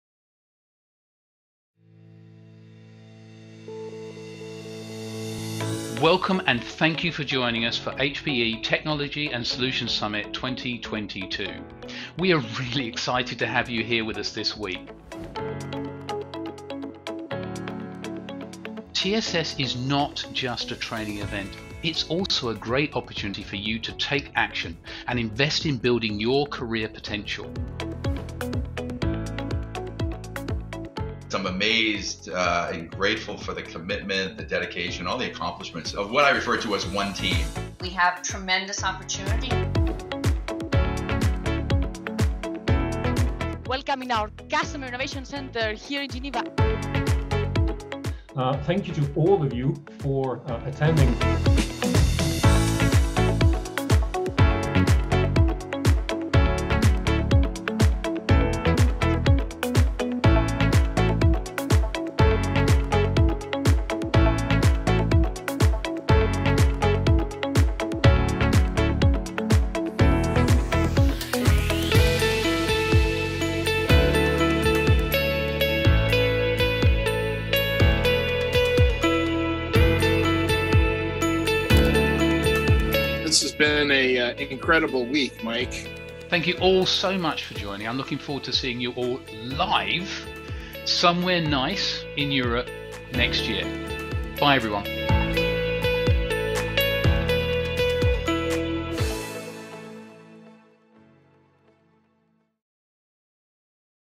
HPE TSS 2022 Highlights
After event video of a technical symposium of HPE named TSS. This year it was a virtual event.

HPE Technology and Solutions Summit (HPE TSS) is a knowledge transfer event targeting Presales Consultants and Solution Architects from both HPE and Partners, covering the full breadth of HPE's product and solution offerings.

Hope to see you all next year in-person!!!